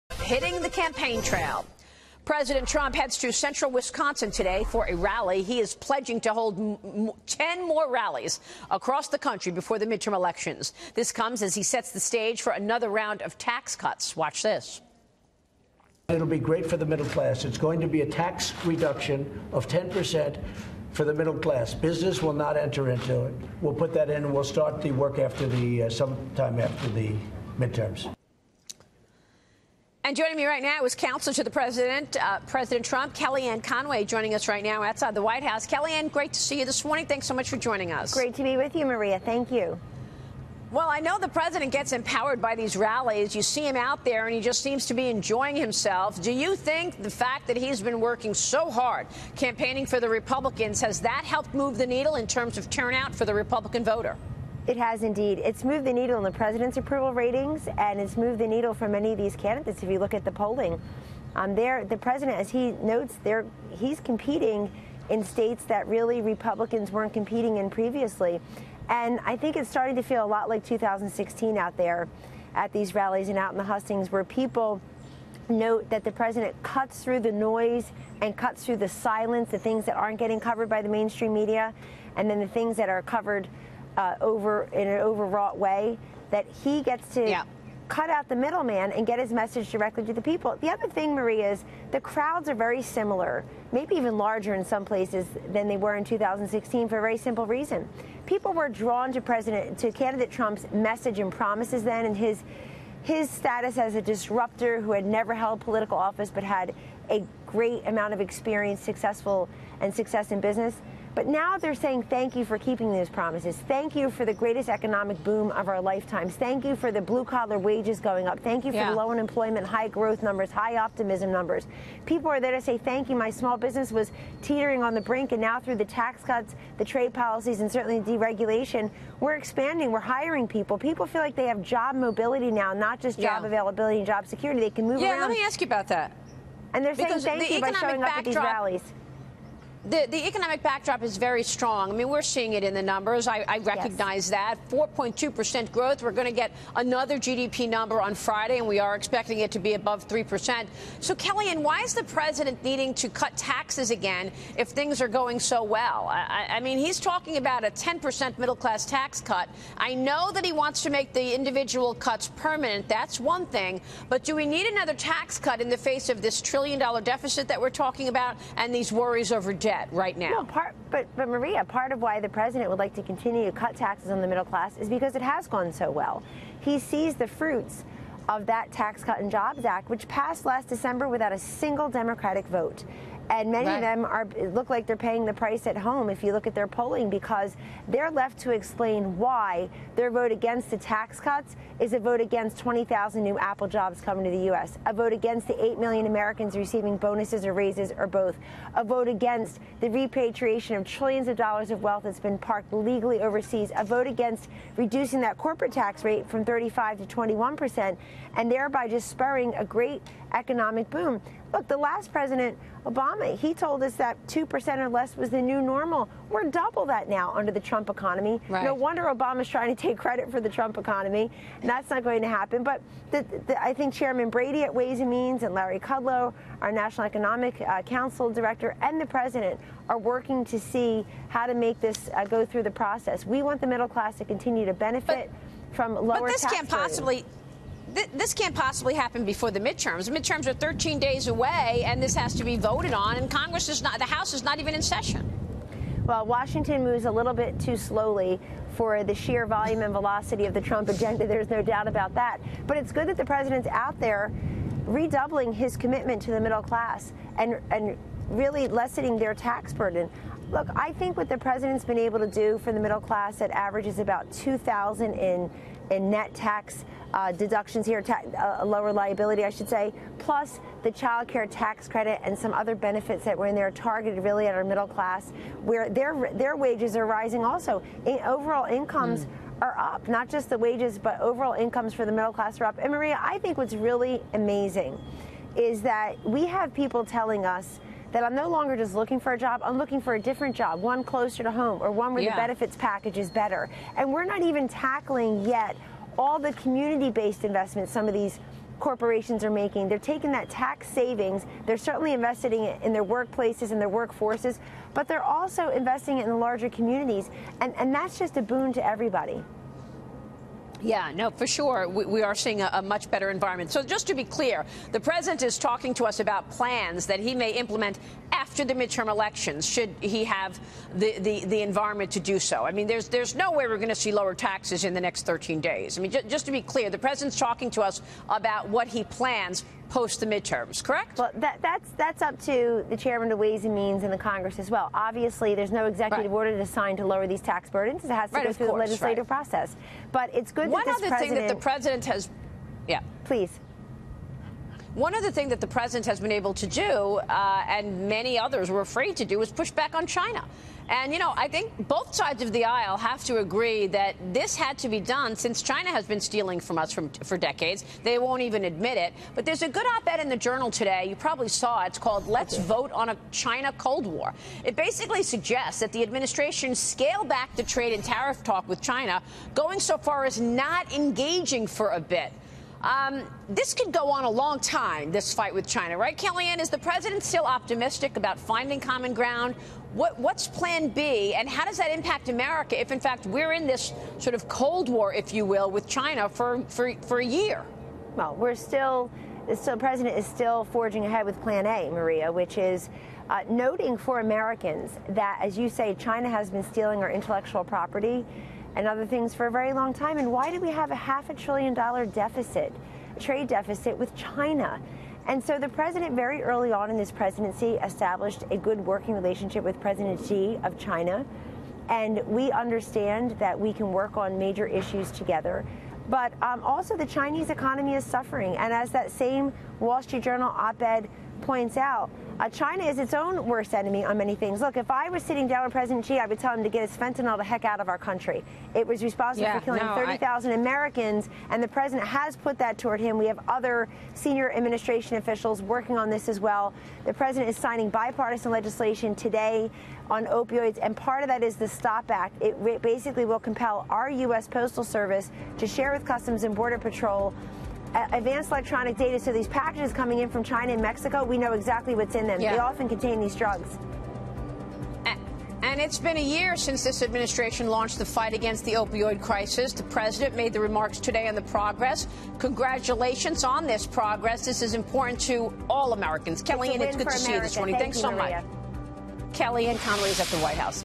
Trump is re-doubling his commitment to the middle class: Kellyanne Conway
Kellyanne Conway, counselor to President Trump, on President Trump hitting the campaign trail for Republican candidates, the state of the economy, Trump's calls for a new round of tax cuts and U.S. trade tensions with China.